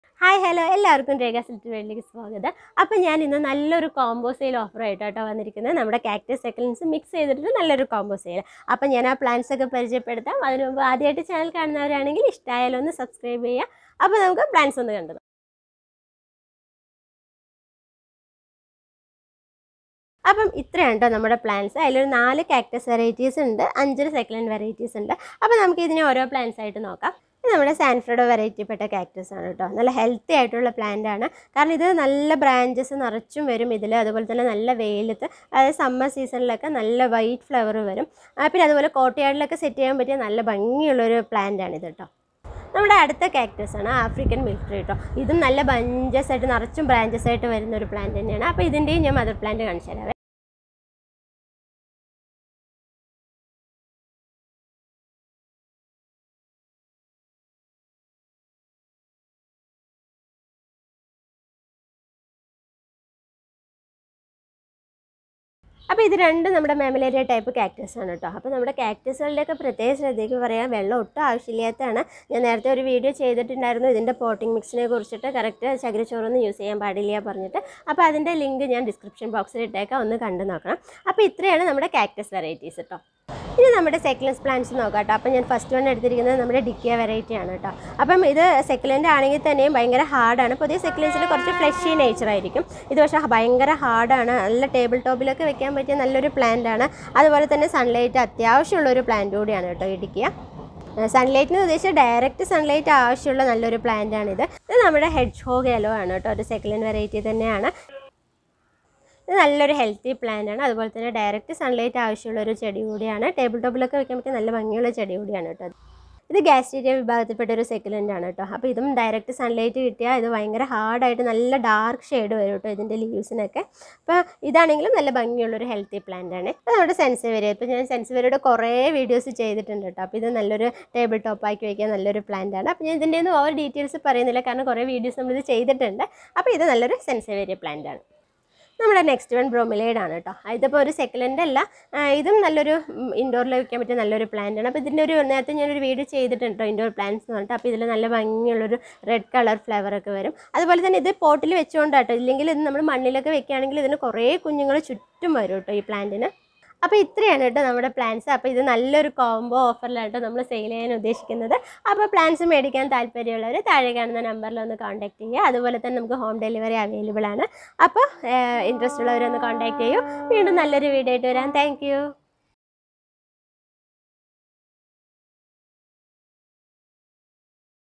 combo sale of cactus & succulents| malayalam| rekha's little world| rekha cr
a combo sale of some healthy cactus and succulents for a affordable price